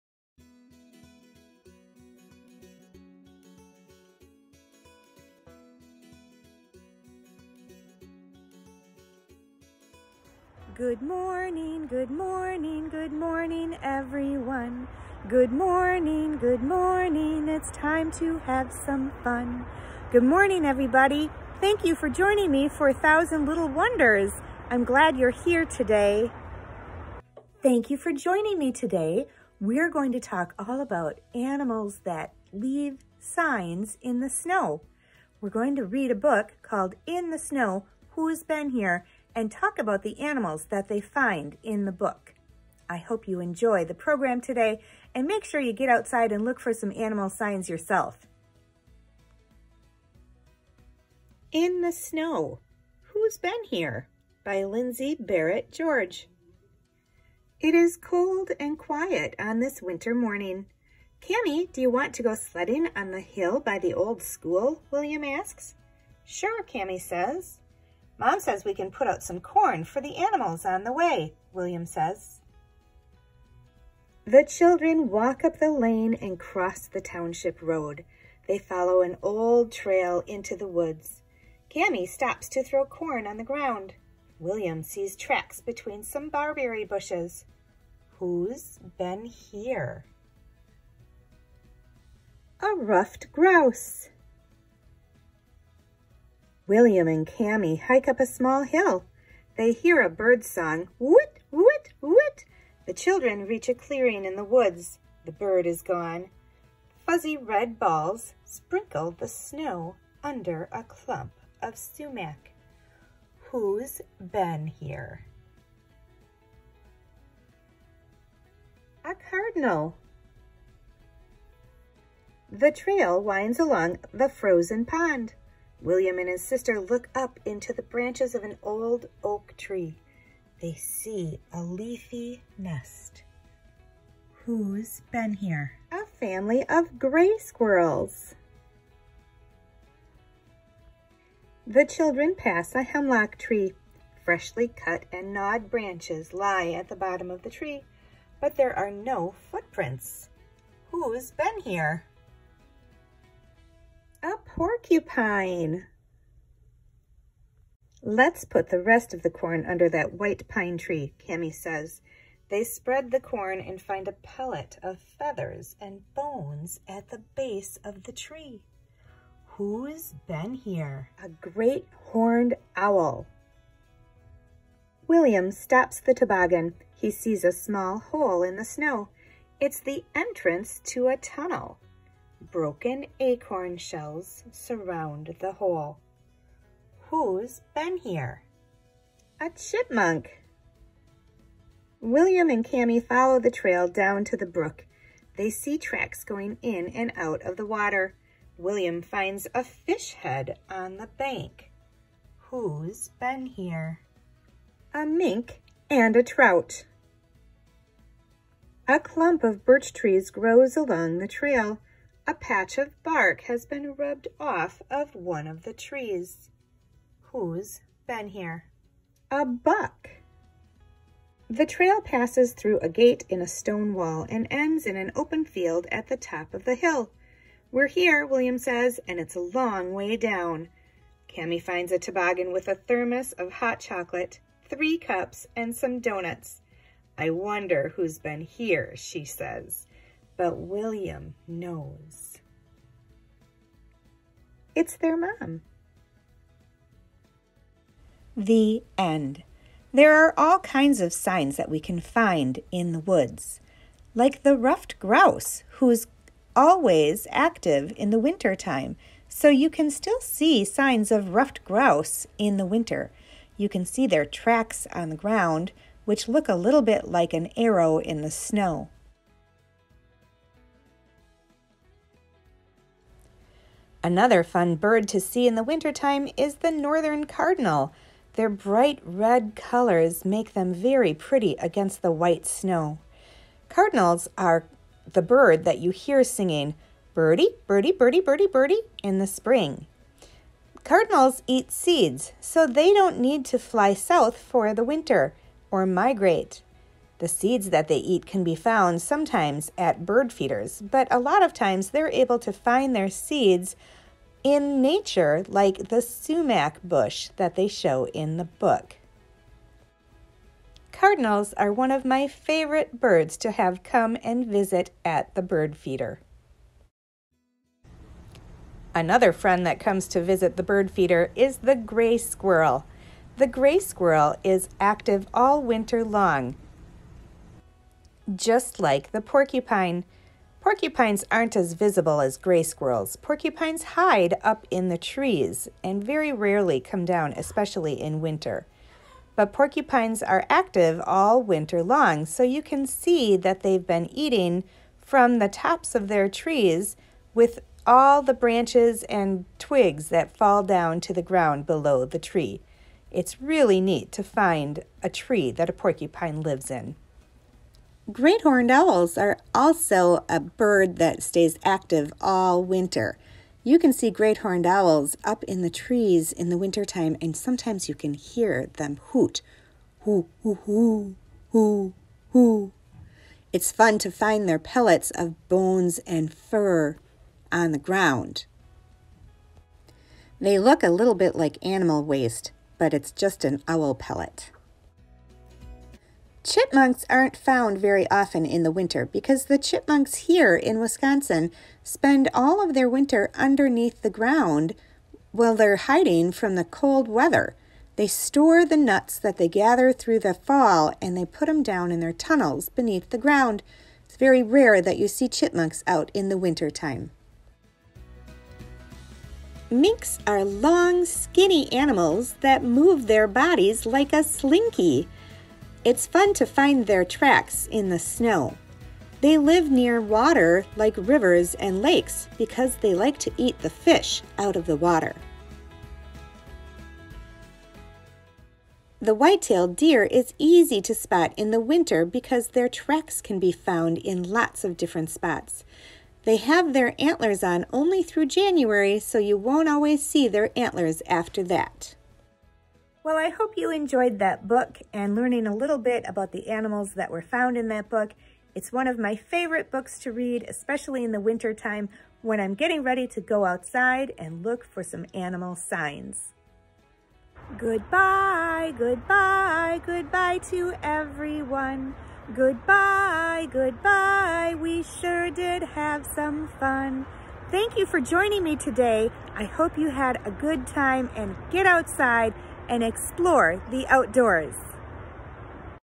1000 Little Wonders - In the Snow: Who's Been Here
Join us for our first Winter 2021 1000 Little Wonders Virtual Program. Read In the Snow: Who's Been Here and learn more about the signs that animals leave behind during the winter with Assistant Naturalist, Stephanie Feuerstein.